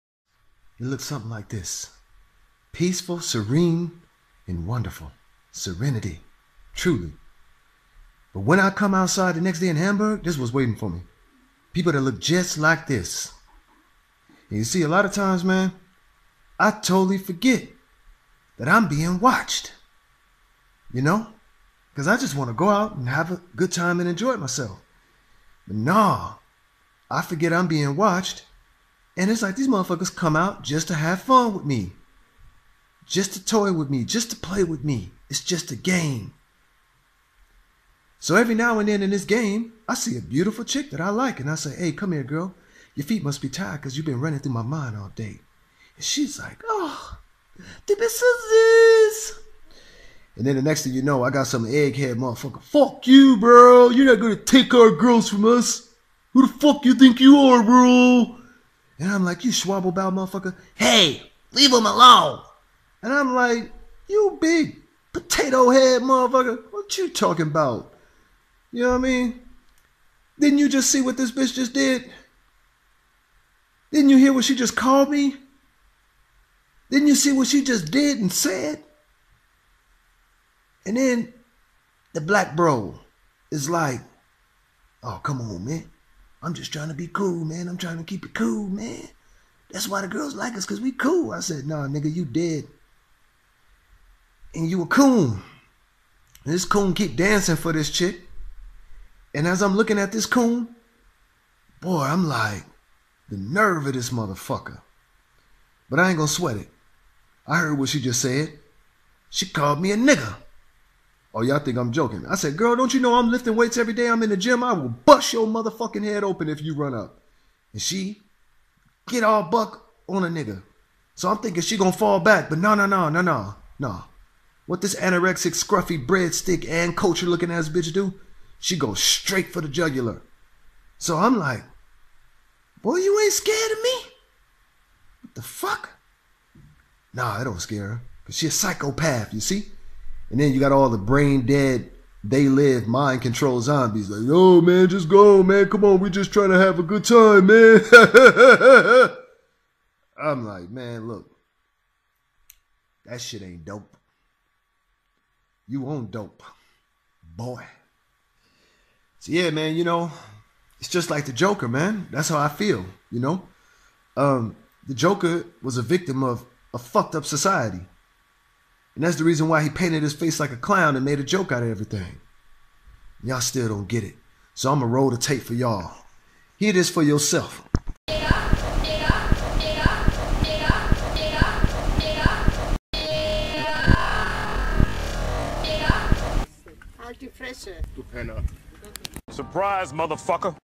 German Girl Called Me a "N*****" in Hamburg
30 years of my life I grew up In Los Angeles never was I ever called a Nigger at any job, school or out in public , yet In Hamburg I"ve been call a N by germans this happen to be at Berliner Tor bahn station at 7:00 pm on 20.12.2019 and if I report This the Stattsanwald and Poliezi will do nothing but call me crazy hatecrimes ACLU this is truely how most germans think about Black People really sick and sad but I just made a joke out of it in this video cause that what most germans are in Hamburg a F ing Joke . Hamburg the worlds most unattrative , boring